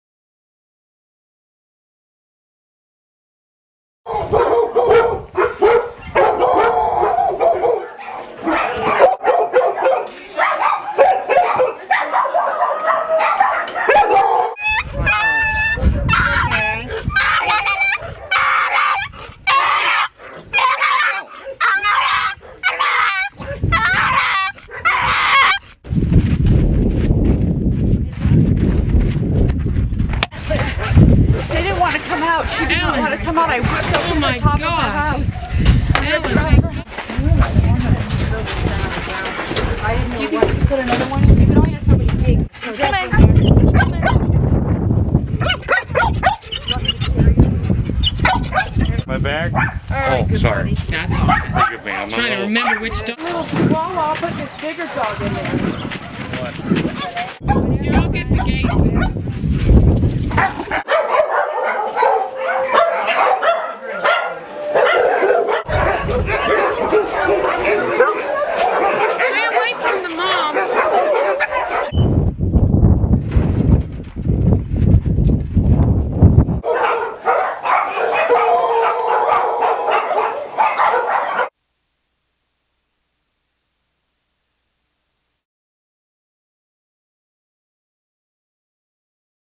Mojave Hoarding
Last Chance for Animals needs your help! We rescued one hundred and eighty so far, there are at least one hundred more to go.
If you are able to foster dogs or cats, vet them, donate food, supplies or transport, we welcome the help!
or The Brittany Foundation and Animal Alliance